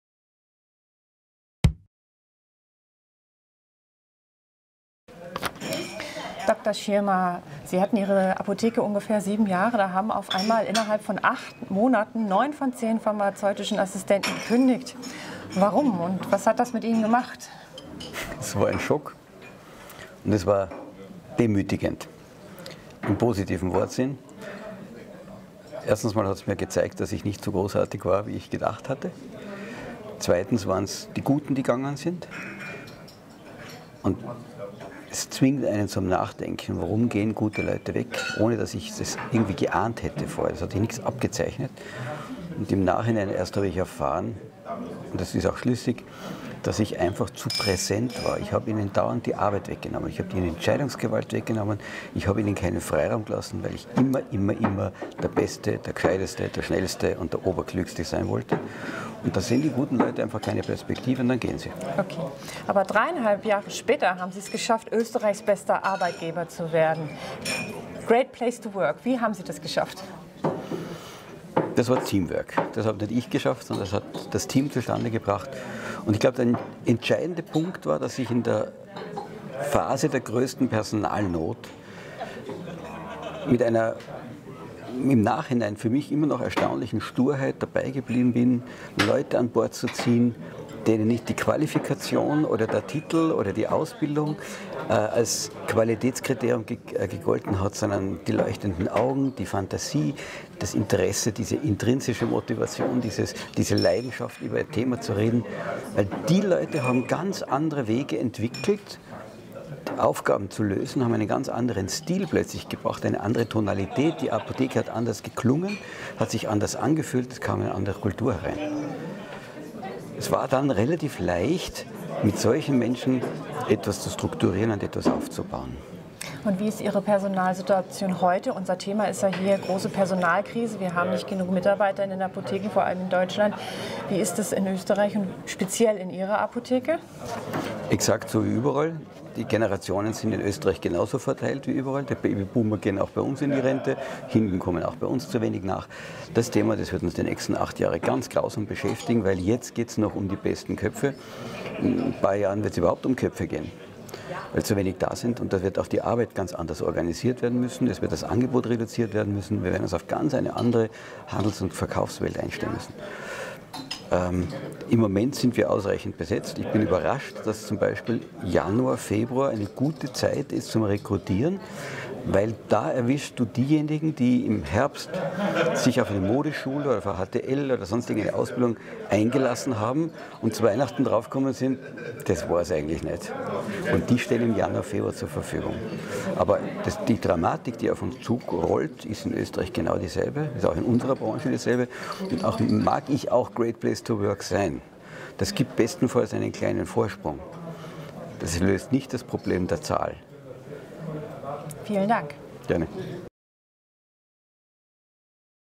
PZ-Managementkongress: Dr. Klaus Schirmer im Interview
Innerhalb von nur acht Monaten kündigten neun von zehn Pharma-Assistentinnen dem österreichischen Apothekeninhaber Dr. Klaus Schirmer. Was hat das mit seiner Apotheke und ihm gemacht? Wie hat er es geschafft, innerhalb von weniger als vier Jahren auf einmal zu einem der besten Arbeitgeber Österreichs, einem „Great Place to Work“ zu werden?